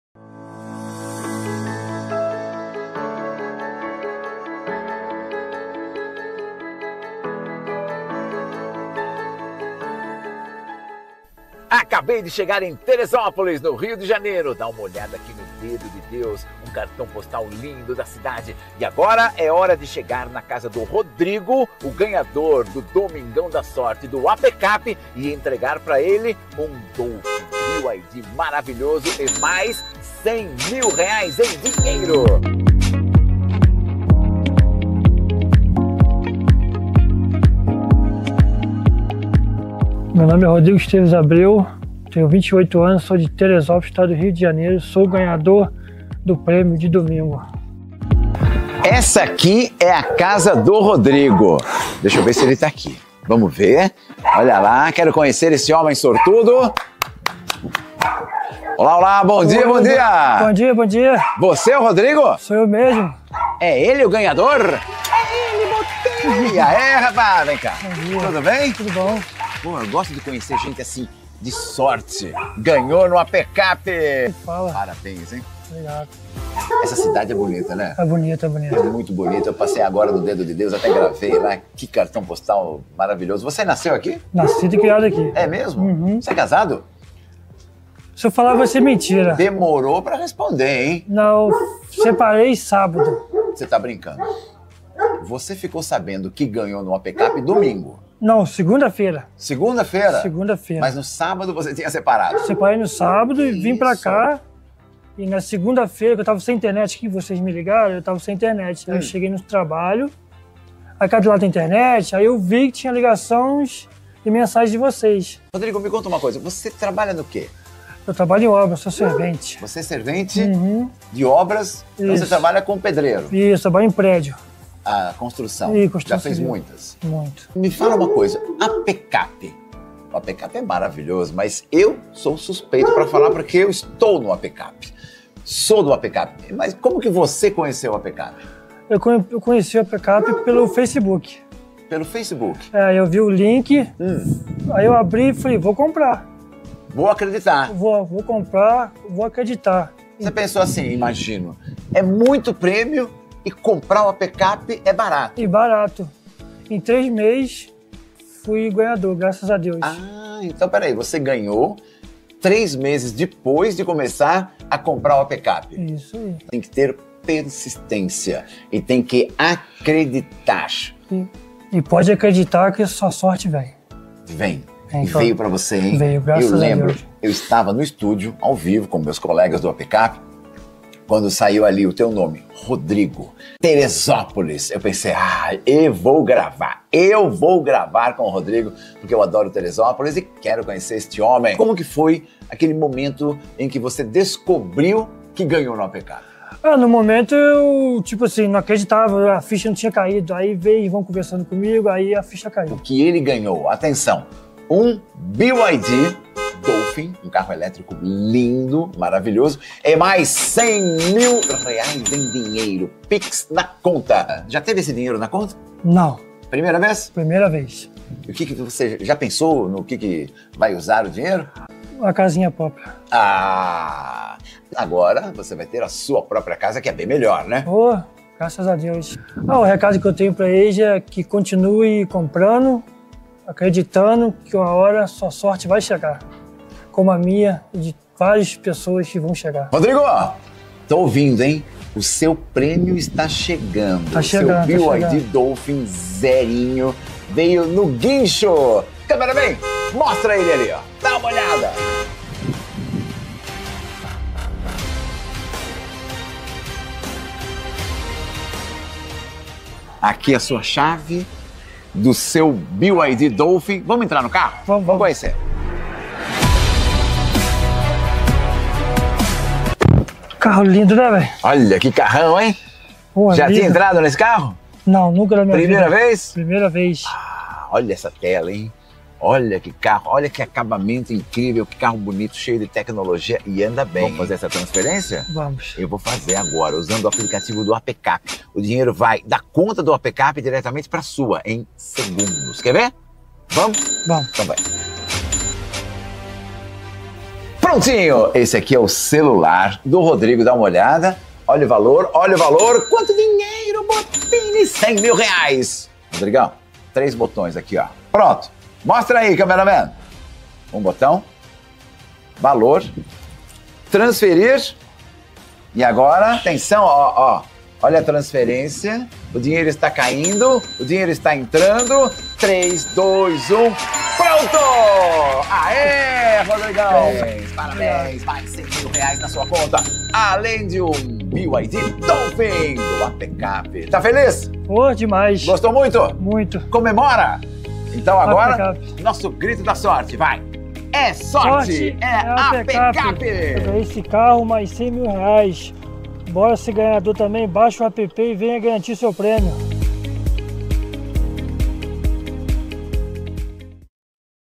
🎉 QUE SORTE, RODRIGO! 🎉
Rodrigo levou sozinho 1 BYD Dolphin + R$ 100.000 no Domingão da Sorte! 🚗💵

A sorte grande sorriu para ele e pode sorrir para você também!

VAI FICAR DE FORA? ACREDITE NA SUA SORTE!

📲 Baixe o app APCAP, compre e participe!